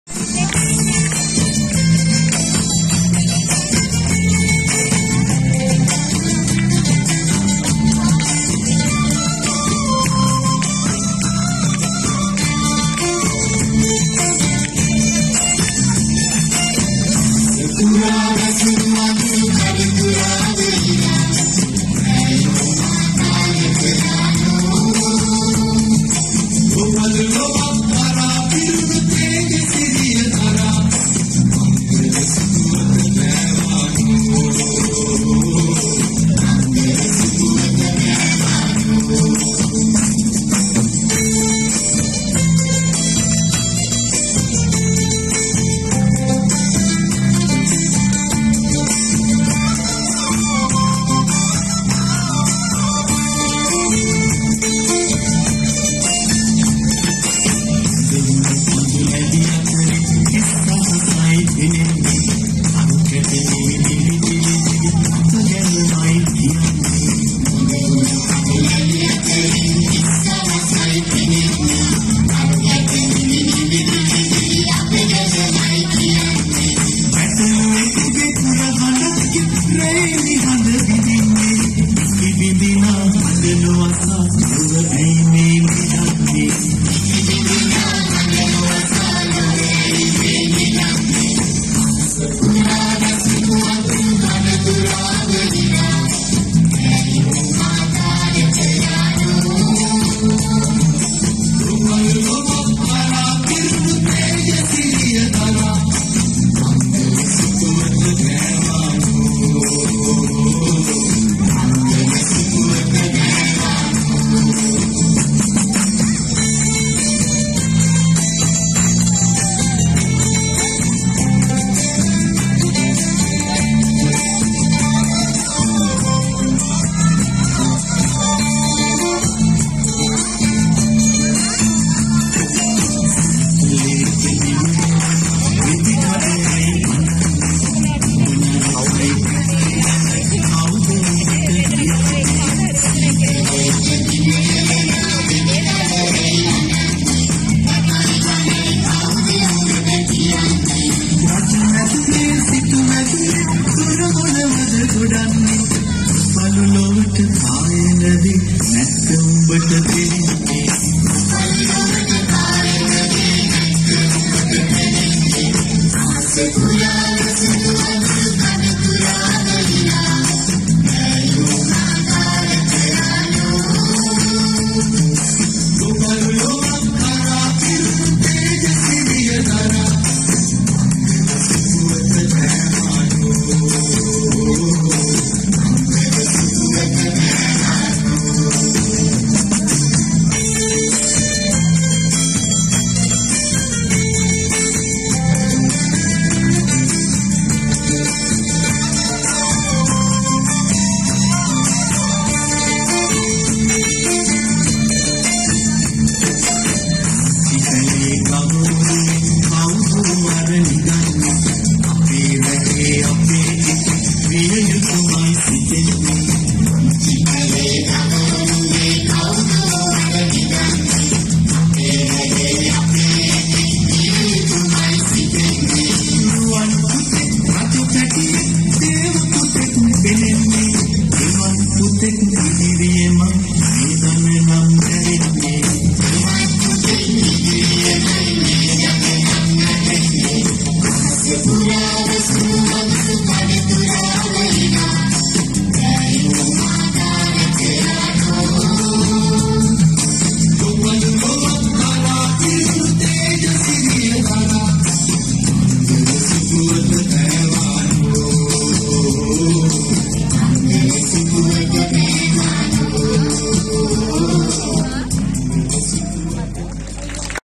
මාරවිල සාන්ත මරියා දහම් පාසැල් වාර්ශික නත්තල් ප්‍රසංගය - ශු. කානික්ක දේව මාතා දෙව් මැදුරේදී -2022
මාරවිල සාන්ත මරියා දහම් පාසැල් වාර්ශික නත්තල් ප්‍රසංගය
මාරවිල ශු. කානික්ක දේව මාතා දෙව් මැදුරේදී පවත්වන ලදී .
Christmas Carols Programme held at Marawila St Mary's Sunday School - 2022 December
SLT මොබිටෙල් ආයතනයේ අනුග්‍රහකත්වයෙන් ඉතා උත්කර්ෂවත් ලෙස පැවැත්වූ නත්තල් විවිධ ප්‍රසංගයේ විශේශ වූ අවස්තා
( Video Clips from  / Miliyansanjeewa Sanjeewa )